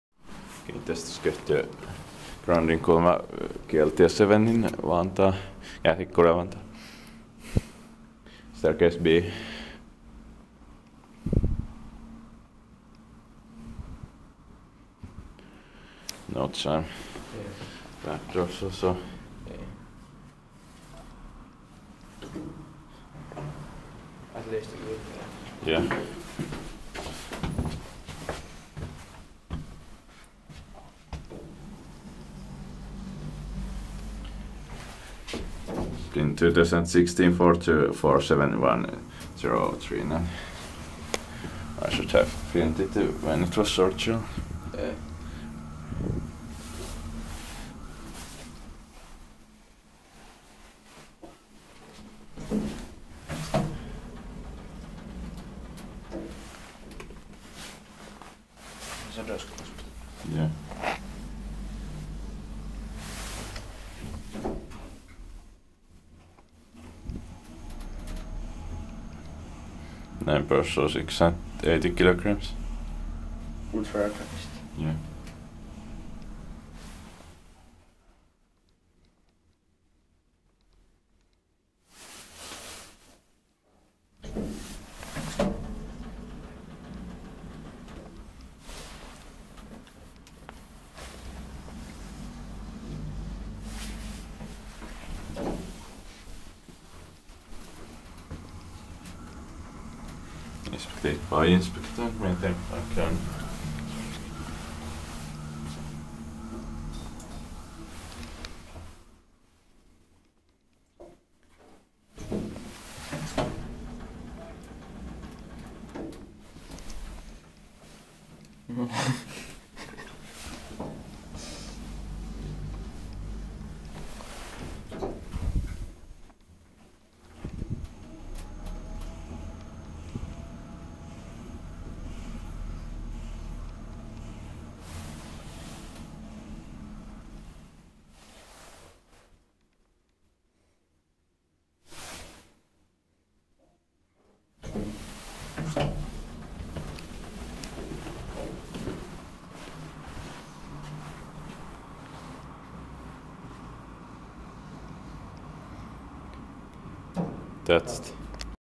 Kone traction elevator/lift in Kielotie 7B, Tikkurila, Vantaa
Location: Kielotie 7B, Tikkurila, Vantaa, Finland
Brand: Kone, Filmed with Finnish Elevators 06
Year installed/modernized: 2016
Serial number: 42471039
Floors: 4(1, 2, 3 ,4)
Capacity: 9 persons or 680 kg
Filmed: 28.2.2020